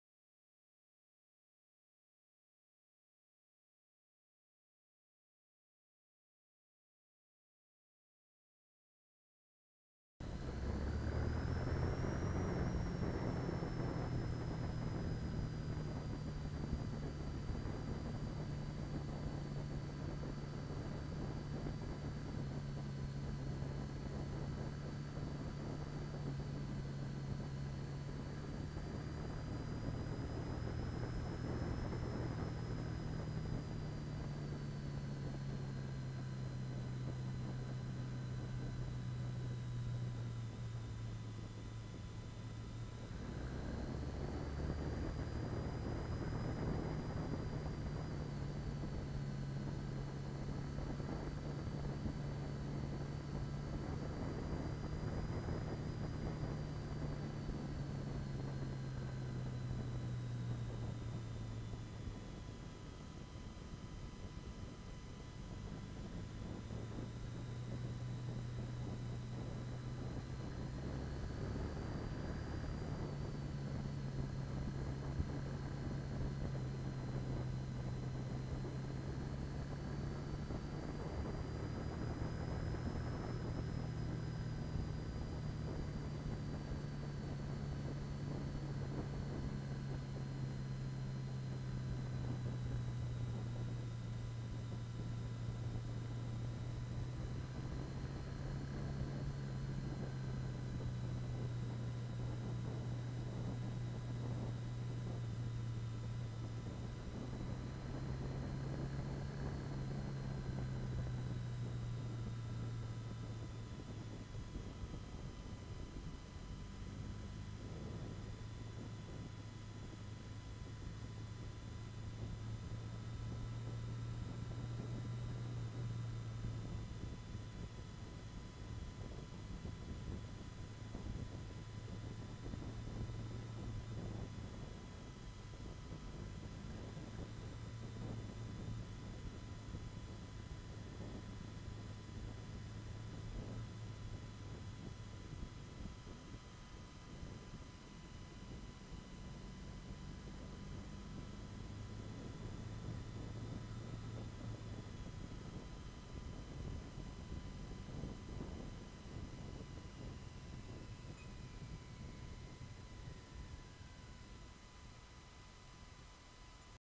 Rick B6105 to Glossop
Rick riding his BMW R1200GS in its natural elements, small twisty Peak District roads.